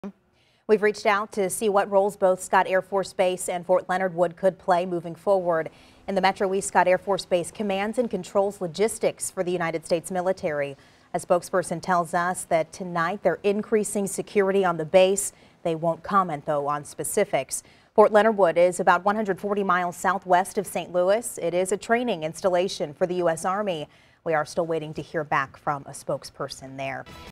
Scott Air Force Base steps up security after Iran missile strike
After Iran struck back at the United States for the killing of a top Iranian general early Wednesday, firing a series of ballistic missiles at two Iraqi bases housing U.S. troops, we reached out to Scott Air Force Base and Fort Leonard Wood to see what roles they could play moving forward.

Get breaking news alerts with the KMOV News app.